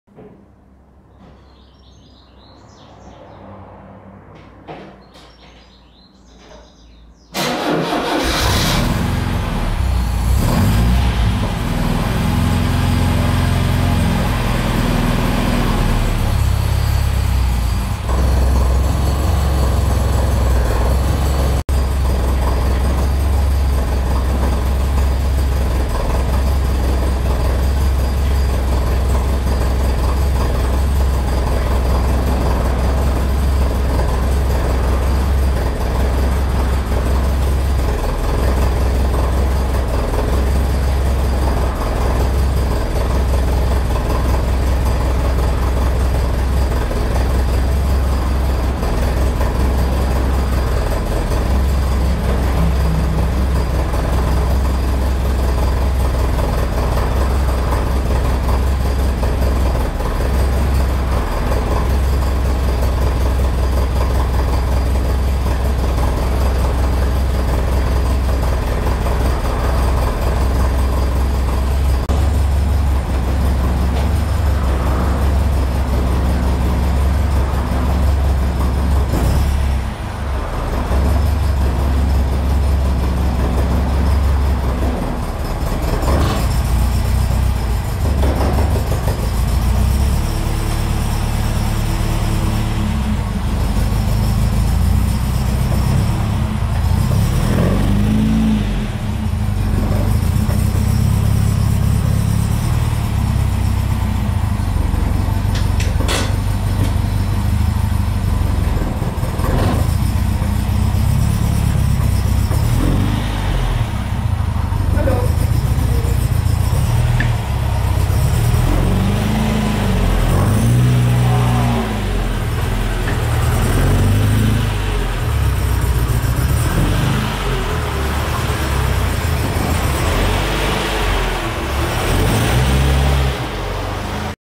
Opel Kadett C Coupe
Mein erstes zusammengeschnittenes Video von meinen Oldtimer der noch keinen TÜV hat .